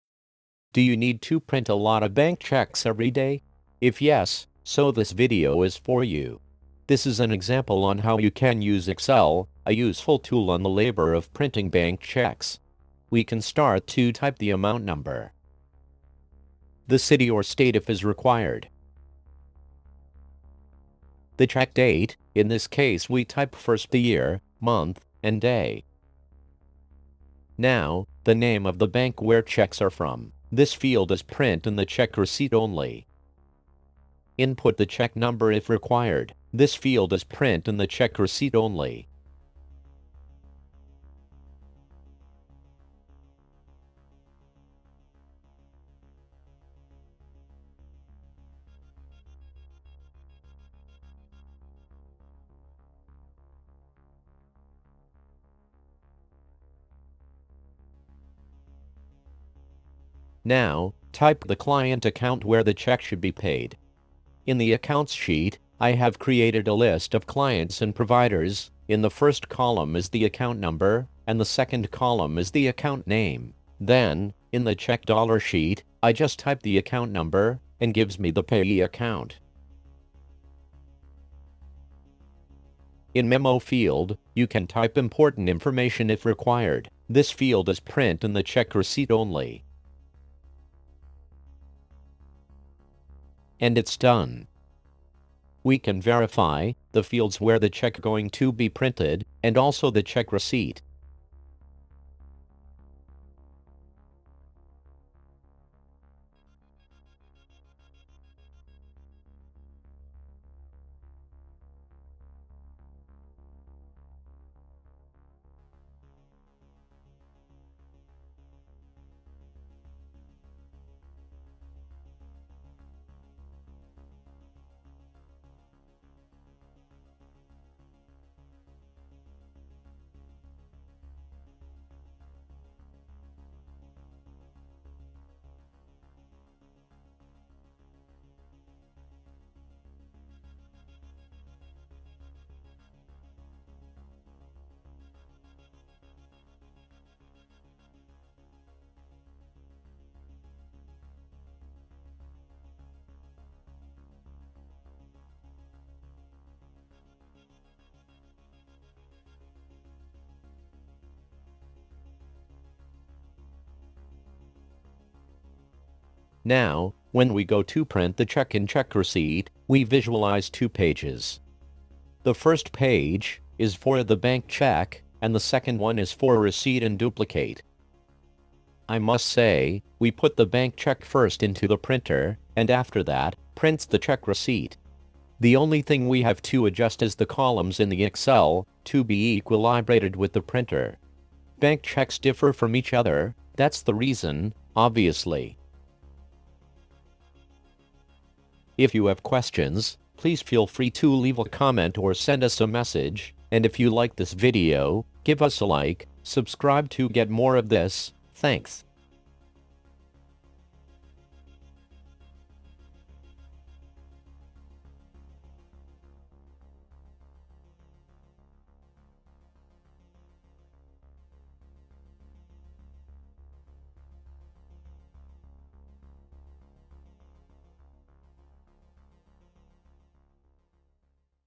Print Bank Checks in Microsoft Excel (Bank Cheque)
Print Bank Checks in Microsoft Excel. Bank Cheque Printing Software that uses any printer available on the market. Created to transpose the value of the full-text check, fill in all the required fields of the bank checks, also printing the receipt sheet. If you need to print bank checks, this tool is for you.

This is a great tool, and enhances the work of any office job.

Print Bank Checks in Microsoft Excel, is a program  that i build with the primary objective to print any bank check, on any printer available on the market, since it’s a Microsoft Excel program. Created to transpose the check amount in full text, completing all required fields in bank checks, and also printing a receipt sheet and its duplicate.

You can use any of your available printer to print cheques. Laserjet, Deskjet, Inkjet, All in One and even Dotmatrix printers. No cumbersome settings and printer handling, you just need to adjust the column and row (simple because, there are no equal stander bank check layout) in print-cheques-excel.xml file to equilibrate with your printer brand.

Easy to Learn. Just 5 Minutes to learn and start using it. Is developed in Microsoft Office, so is easy even for novoice computer users. This great Excel file enhances the work of any office job.

We can start by typing the amount, the city or state if required, the cheque Date (type first the year, month, and day).

Next type the name of the bank where cheques are from, and type the Account code number from where the cheque should be Transfer/deposited.

So when you type the account number, returns the Payee account Name.

In the Accounts sheet, is where you find the sheet list of Clients and Providers. The first column is the Account Number, and the second column is the Account Name.

Next on the Memo field you type important information if required, and it’s done.

You can verify the fields where the cheque going to be printed (layout of the cheque receipt). Gives you two pages, the first page is the bank check it self, and the second is the receipt (duplicate).

First put the bank cheque on the printer, and after the bank cheque comes out, the check receipt sheet’s are printed (load your printer if is out of paper).

Adjust the columns and rows in the print-cheques-excel.xml to be equilibrated with the printer. Bank cheques differ from each other, that’s the reason to adjust the columns and rows in Excel.

If you use more than one kind of check layout, it’s always possible to create/duplicate a new file or sheet tab to different check layouts (always of course the Account code number must be synchronized with the Accounts Sheet).